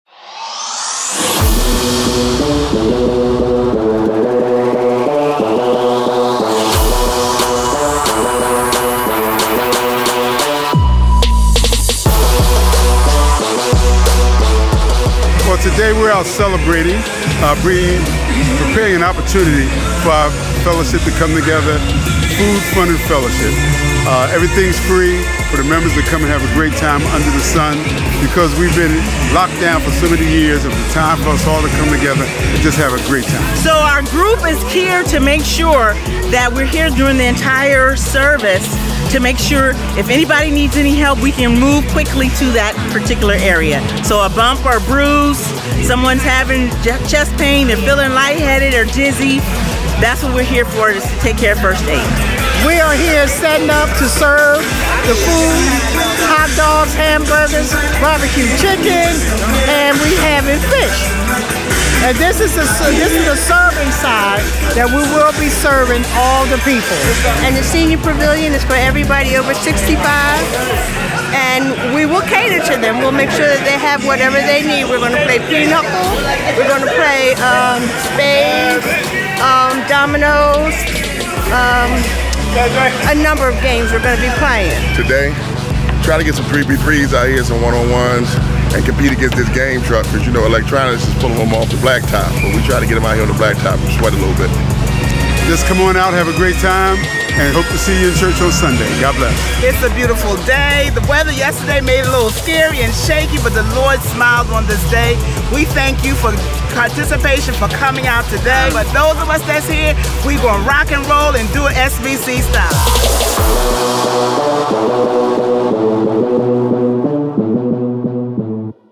🎉🍔🎊 Cookout Recap at Sharon Baptist Church! 🎊🍔🎉
We had an amazing time at our Cookout hosted by Sharon Baptist Church! 🌞🍗🍔 Members came together for a day of fun, fellowship, and delicious food. Check out the video recap to see all the excitement!
Thank you to everyone who made this day possible. 🙏 A special shoutout to the Governor for gracing us with his presence. 😊